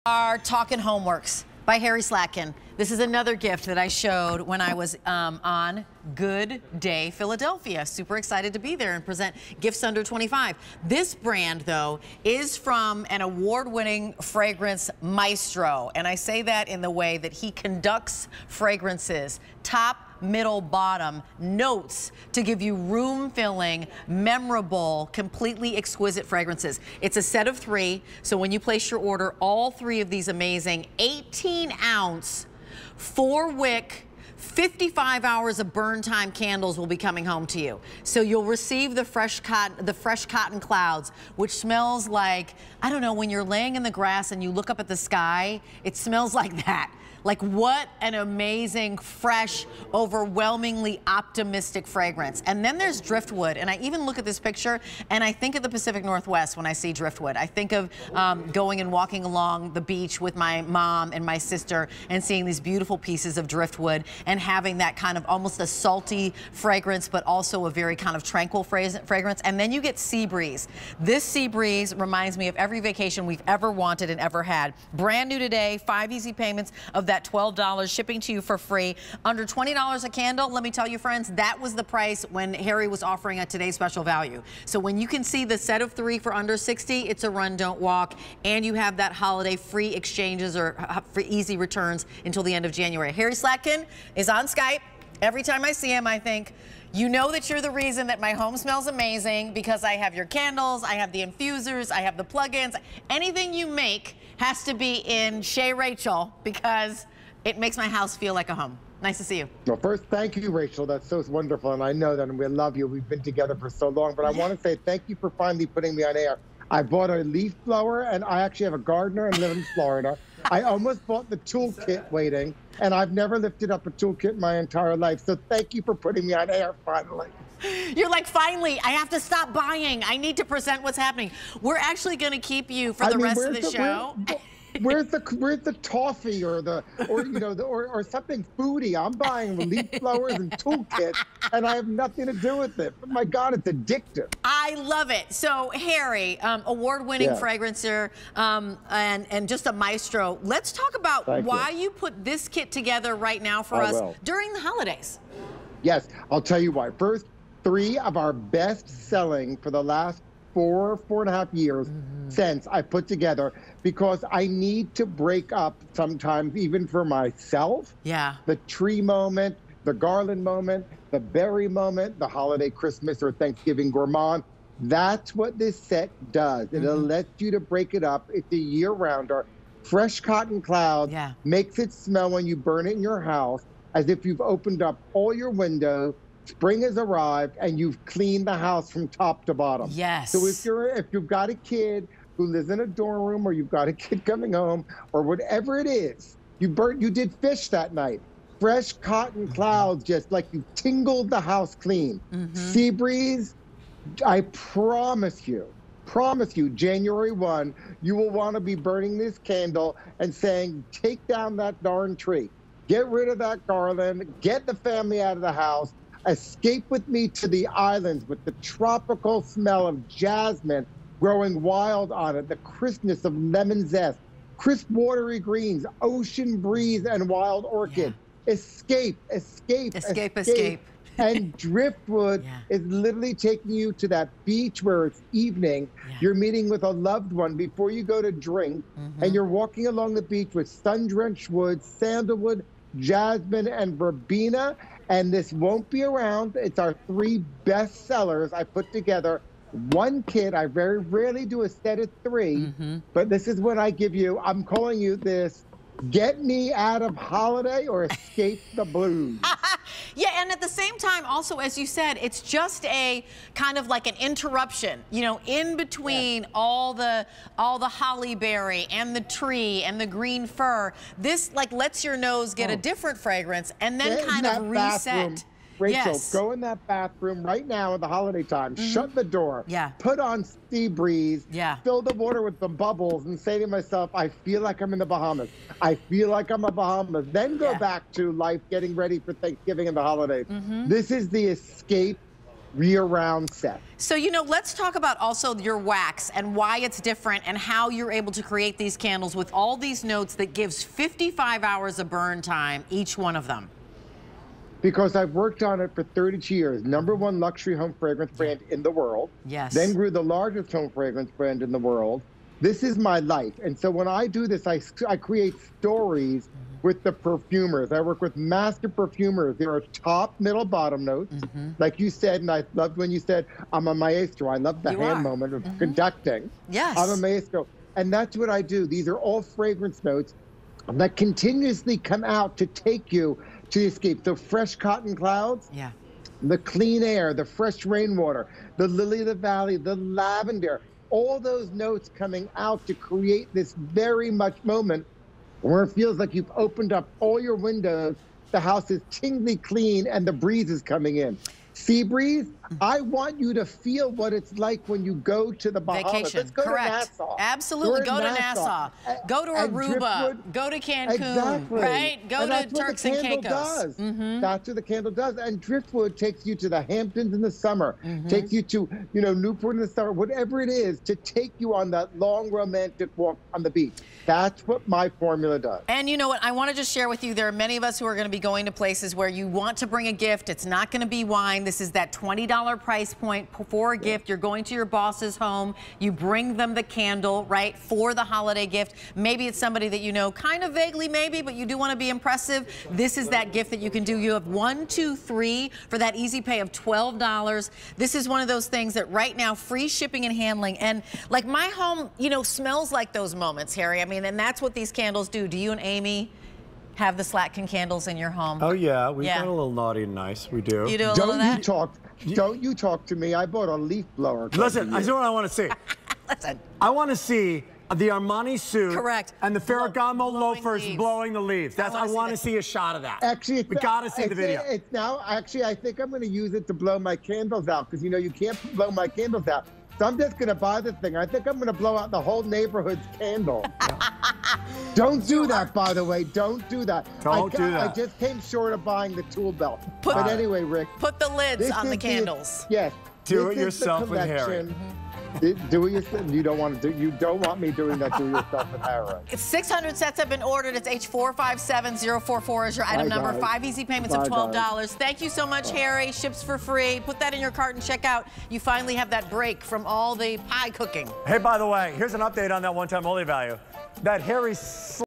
HomeWorx by Slatkin & Co. Set of 3 18oz Fresh Candles on QVC
HomeWorx by Slatkin & Co. Set of 3 18oz Fresh Candles

Create company-ready radiance with the four-wick flicker and fresh fragrance of these candles. Fresh Cotton Clouds, Sea Breeze, and Driftwood scents emanate from the decorative glass containers for an instant room refresh. From HomeWorx by Slatkin & Co.